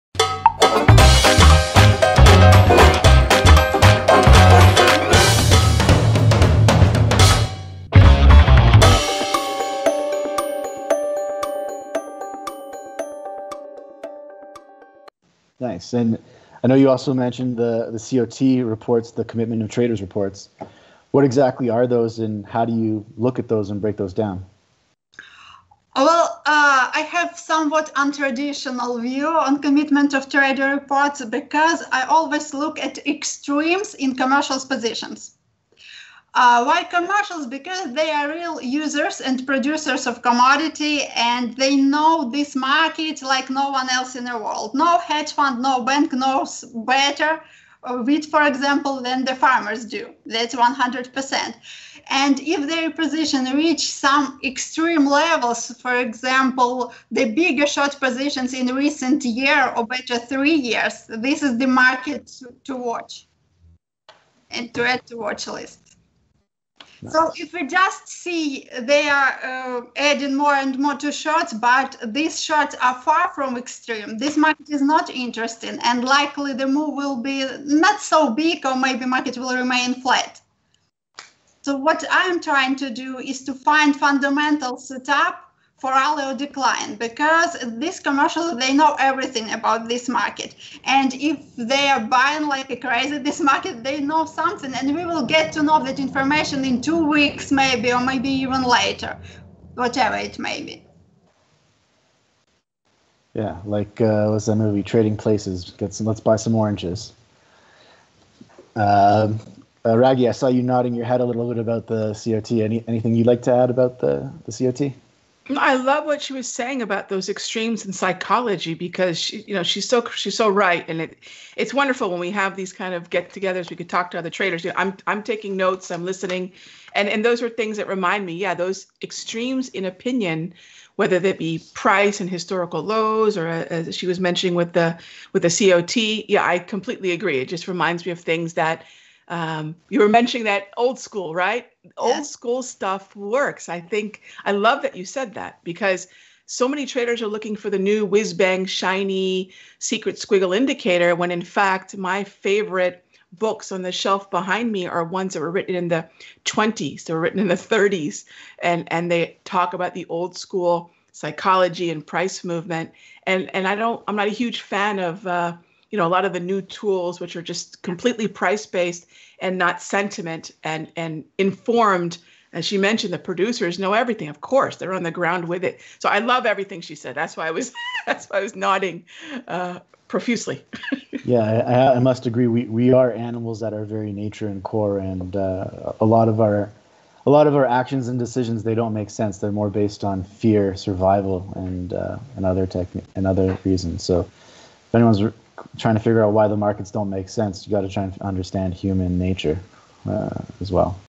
Inna Rosputnia: Old school stuff works!
In Episode 10 of Trading WTF, Inna Rosputnia explains why she uses the commitment of traders indicator and relies on decades-old analysis techniques. Raghee Horner concurs, adding that old school stuff works. Her library is filled with books from the ‘20s and ‘30s.